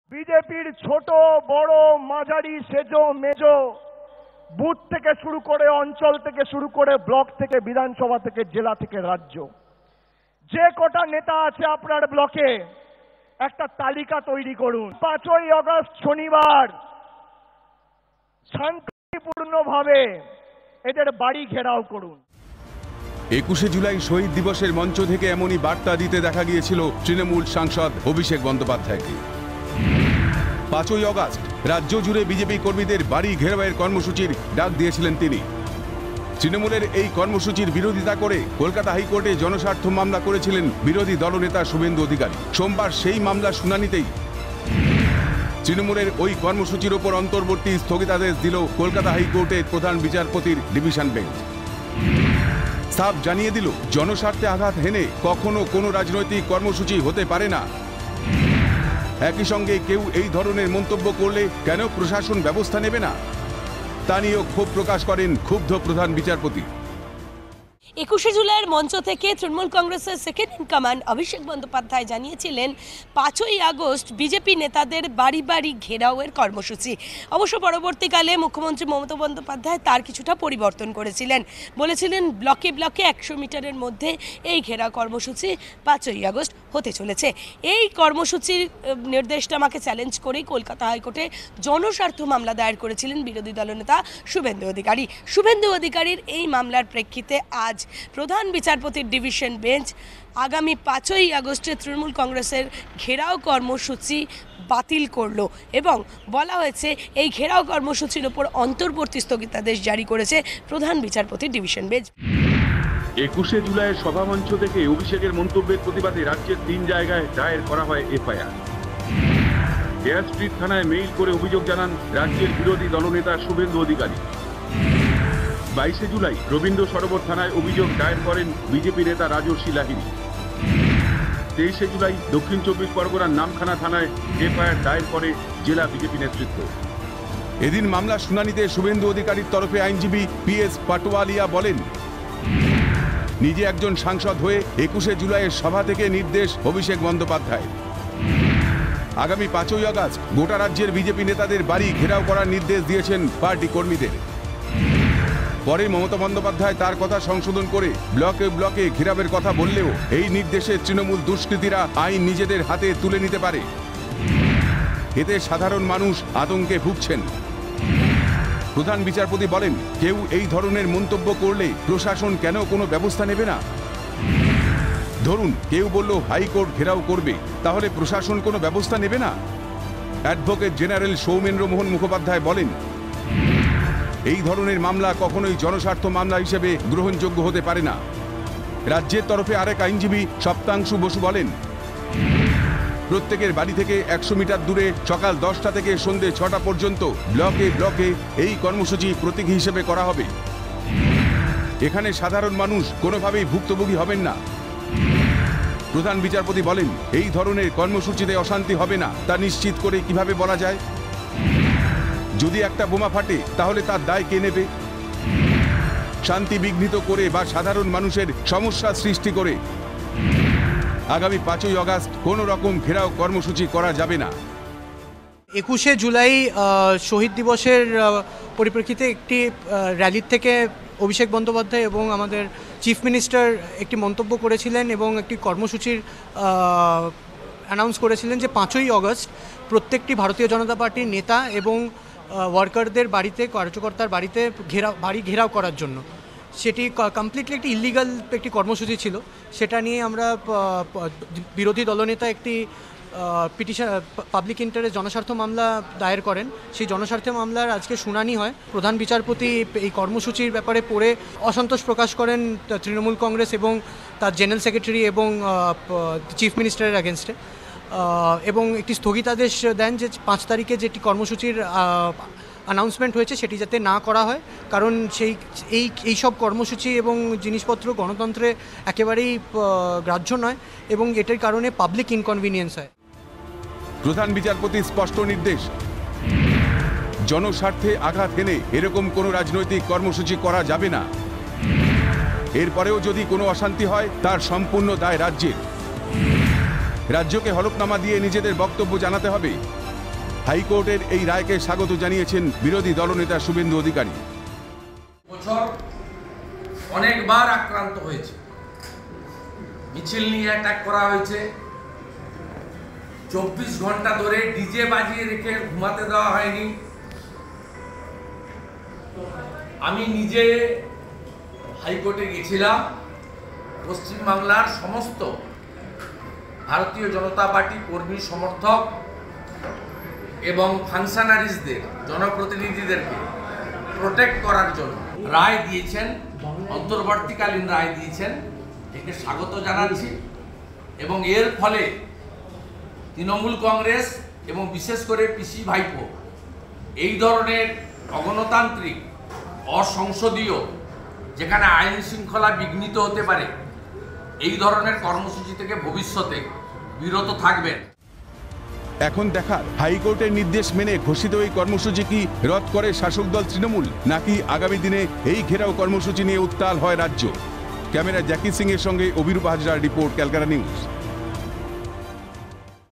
বাড়ি ঘেরাও কর্মসূচিতে অন্তর্বর্তী স্থগিতাদেশ কলকাতা হাইকোর্টের | ক্যালকাটা নিউজ
বাড়ি ঘেরাও কর্মসূচিতে অন্তর্বর্তী স্থগিতাদেশ কলকাতা হাইকোর্টের

কেন্দ্রীয় বঞ্চনার প্রতিবাদে ৫ আগস্ট রাজ্য জুড়ে বিজেপি কর্মীদের বাড়ি ঘেরাওয়ের নির্দেশ দিয়েছিলেন তৃণমূল সাংসদ অভিষেক বন্দ্যোপাধ্যায়। এই কর্মসূচির বিরোধিতা করে হাইকোর্টে জনস্বার্থ মামলা করেছিলেন বিরোধী দলনেতা শুভেন্দু অধিকারী। সোমবার মামলার শুনানিতে ওই কর্মসূচির ওপর অন্তর্বর্তী স্থগিতাদেশ দিল কলকাতা হাইকোর্টের প্রধান বিচারপতির ডিভিশন বেঞ্চ।